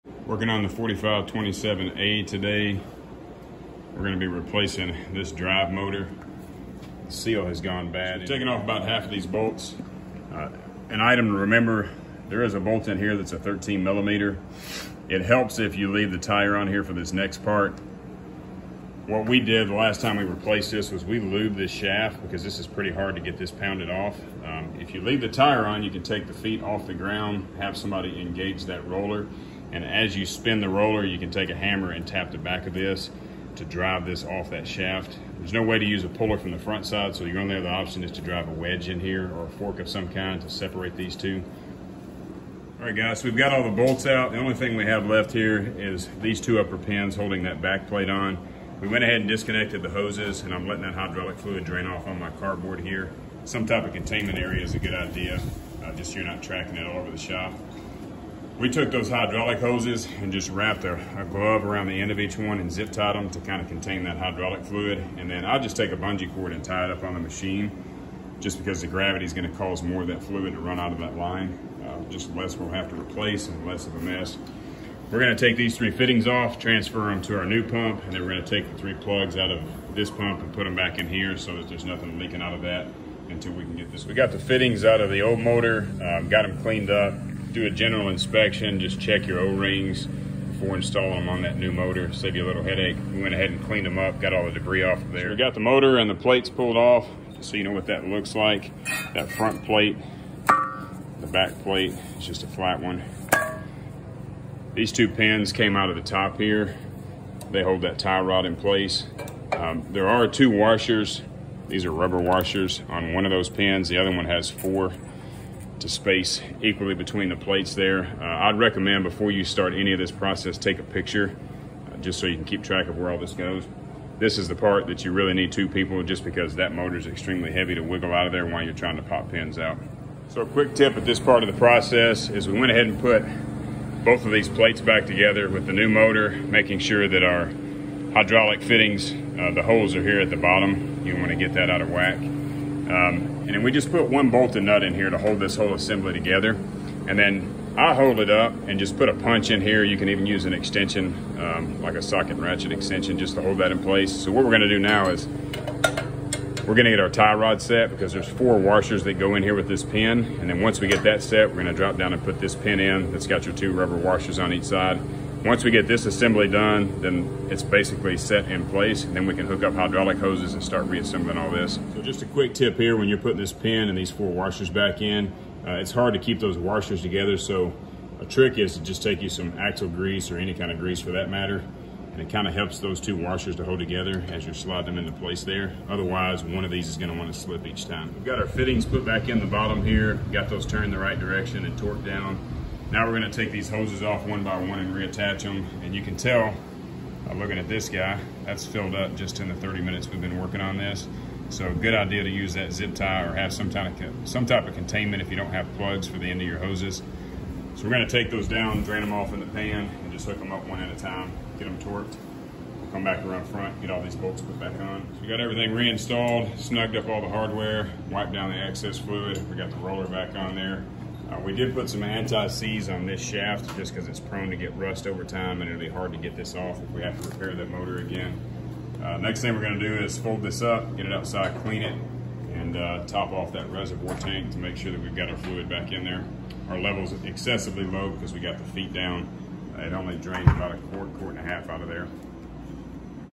Haulotte 4527A Drive & Set - Hydraulic drive motor leak and repair
Full process to change out a drive motor on a drive and set towable articulating man-lift